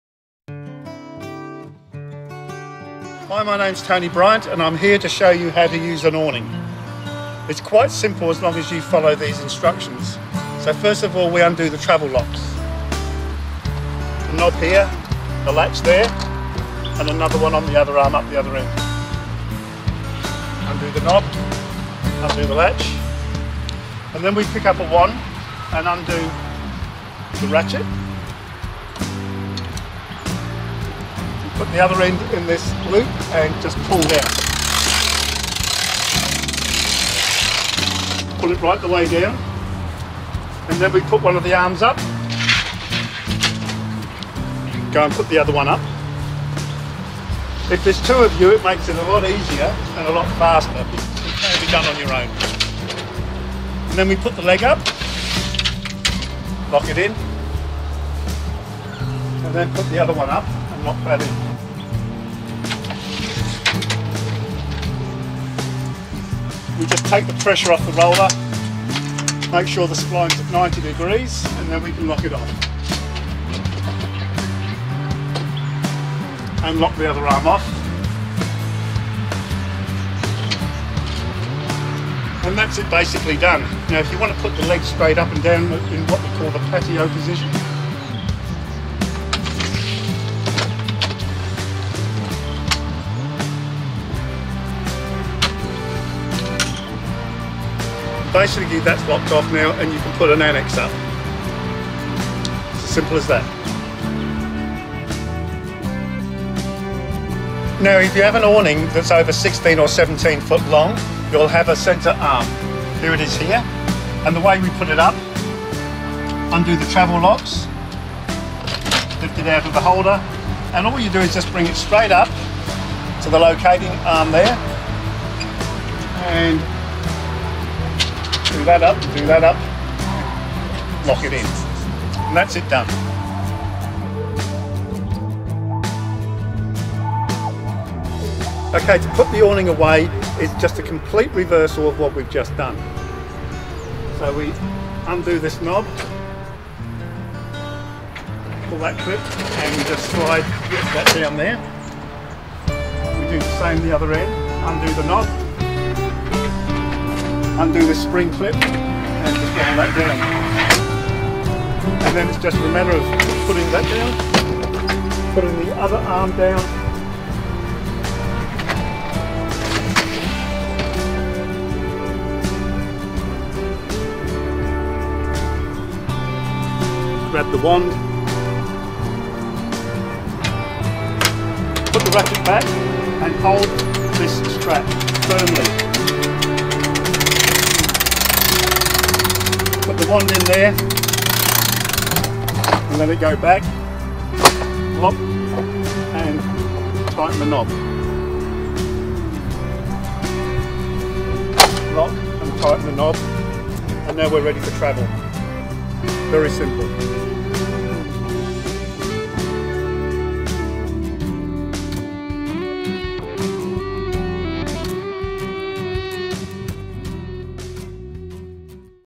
Setting Up A Royal Flair Caravan Awning
RV Guru Tony Briant is setting up a Royal Flair Roll - Out Awning. This short video shows instructions and gives a step by step guide to help anyone erect and close the Roll - Out Awning.